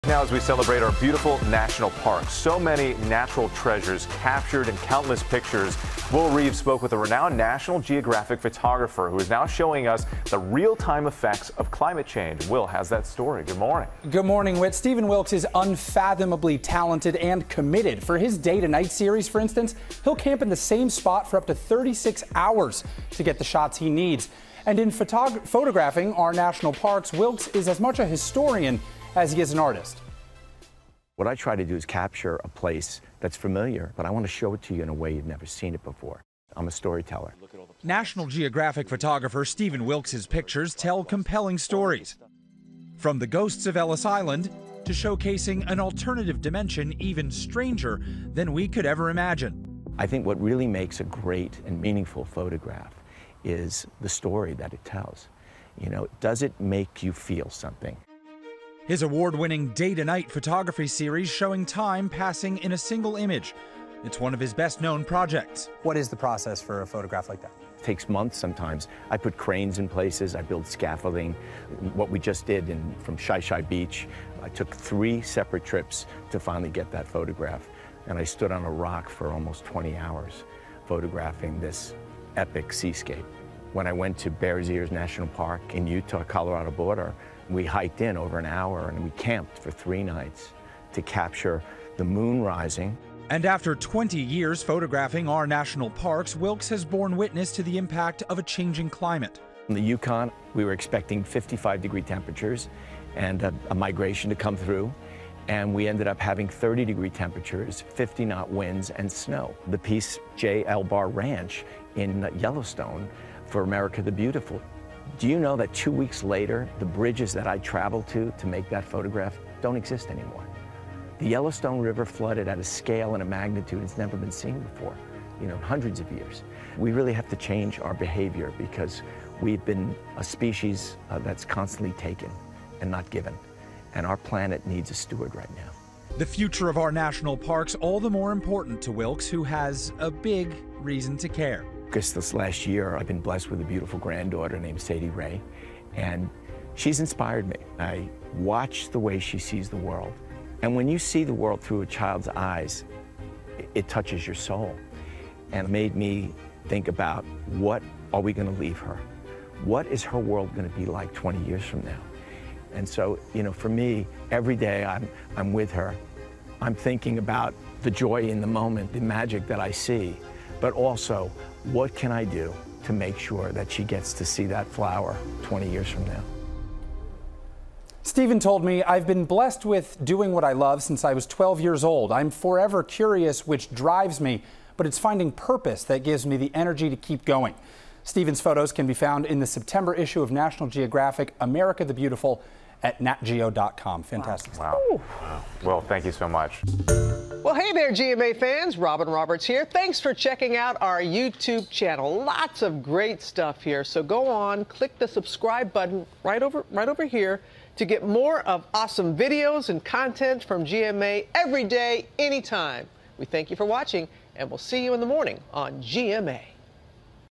National Geographic photographer's pictures document effects of climate change l GMA
Stephen Wilkes has been photographing national parks for more than 20 years and his newly born granddaughter inspires him to continue bearing witness to how they're affected by climate change.